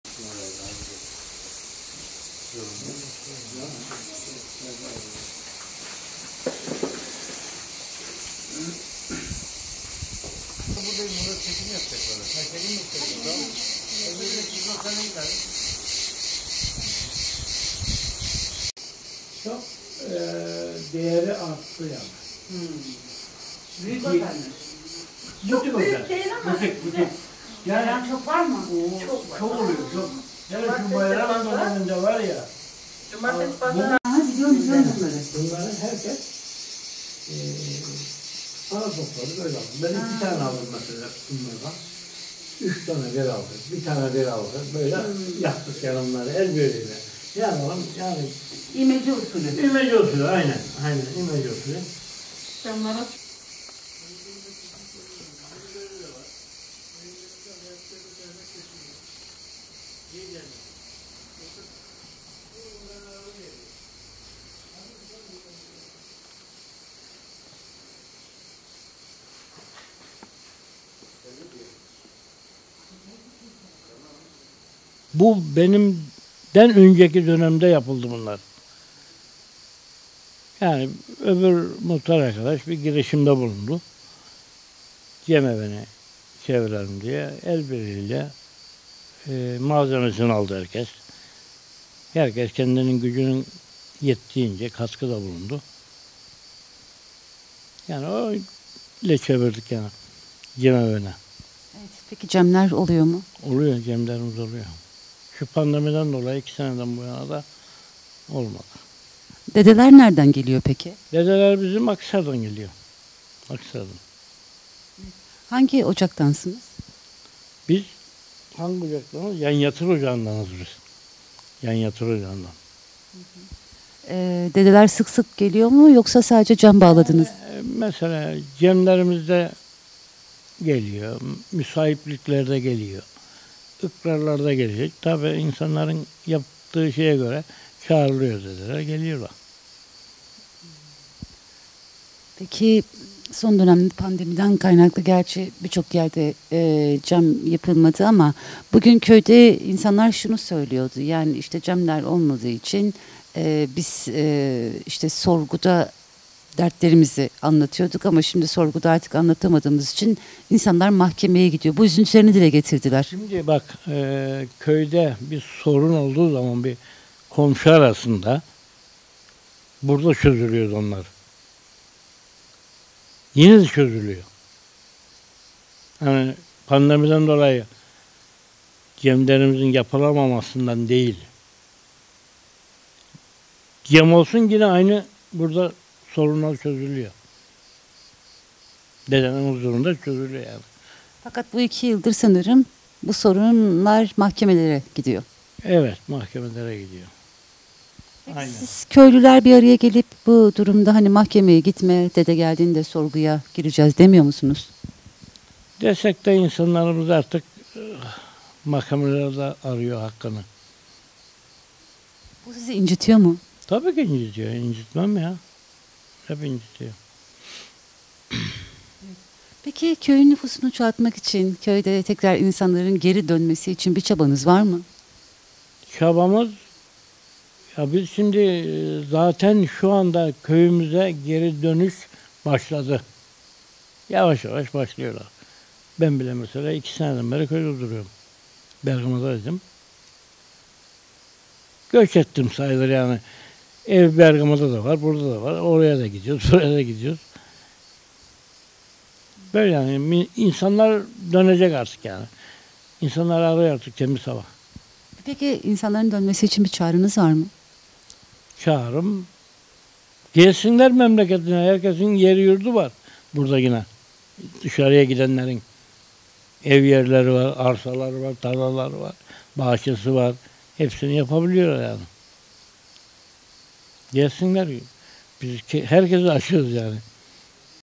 Bergama Kapukaya köyündeki atıl durumdaki okul cemevine dönüştürüldü
PİRHA- İzmir Bergama’ya bağlı Kapukaya köyünde atıl durumda olan eski bir okul köylülerin emeği ile cemevine dönüştürüldü.